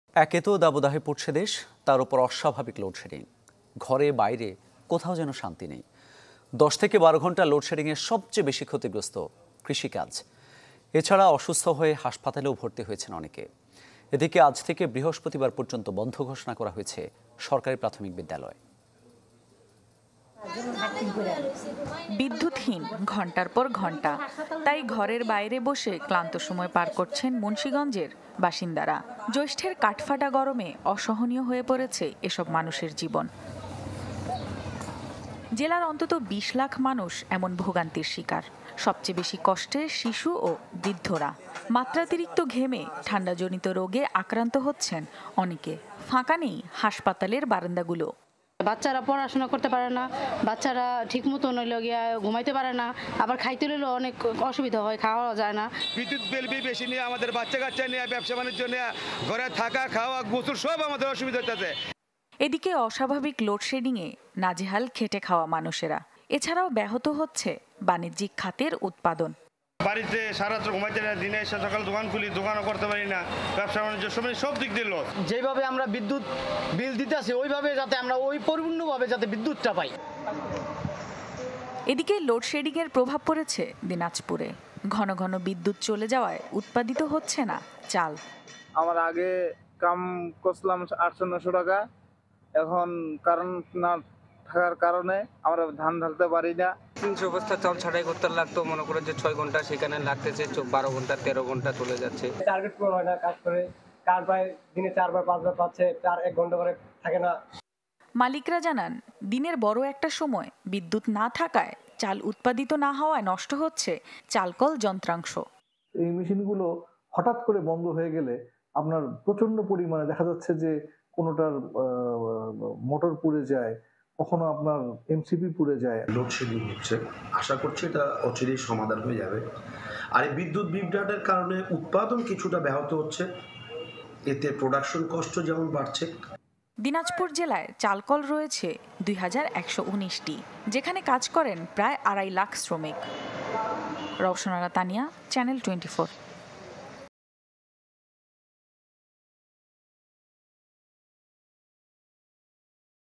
খেটে খাওয়া মানুষ থেকে মিল মালিক; লোডশেডিংয়ে ভালো নেই কেউ | Loadshedding | Channel 24
খেটে খাওয়া মানুষ থেকে মিল মালিক; লোডশেডিংয়ে ভালো নেই কেউ

»» Office Address:
Bangladesh.